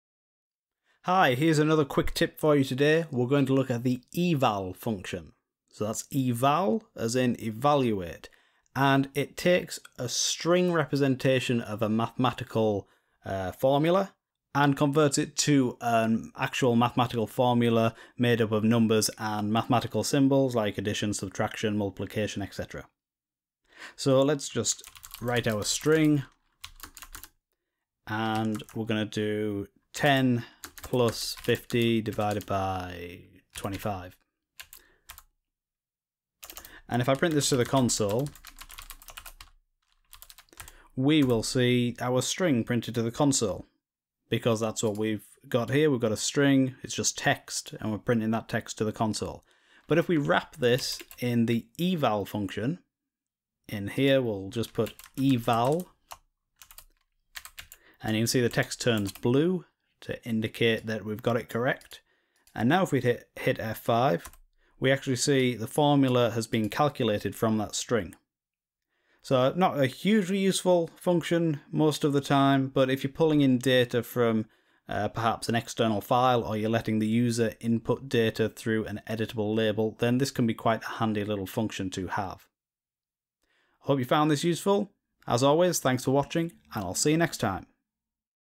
How to use the eval function in HISE
How to evaluate strings in HISE.

Get HISE - hise.audio/